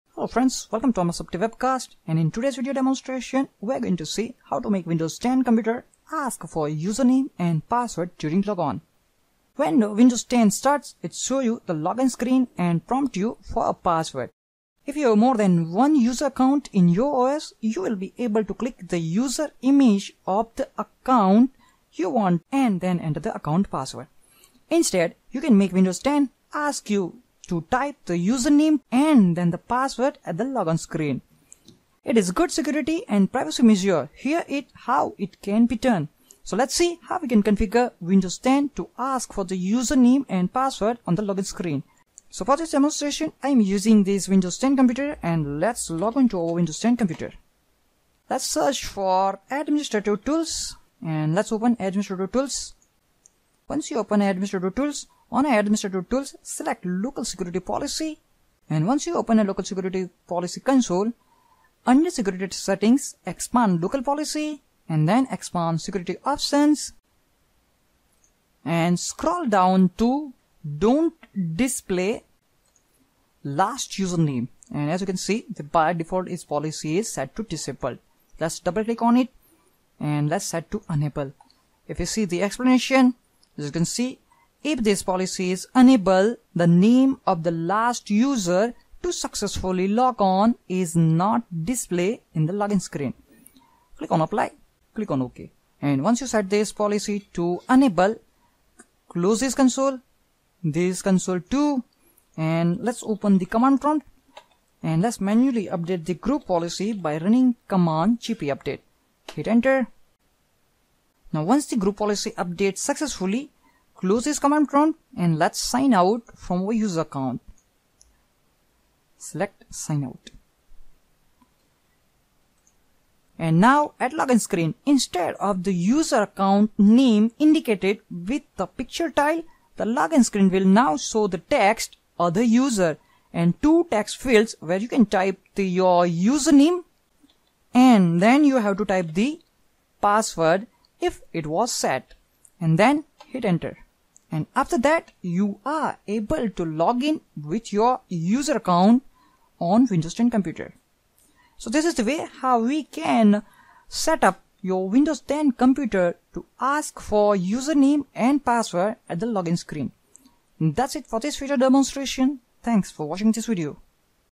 How to Make Windows 10 PC to ask Username and Password at Login Screen
In this video i will show you How to make Windows 10 ask for user name and password at Login Screen on windows 10 PC.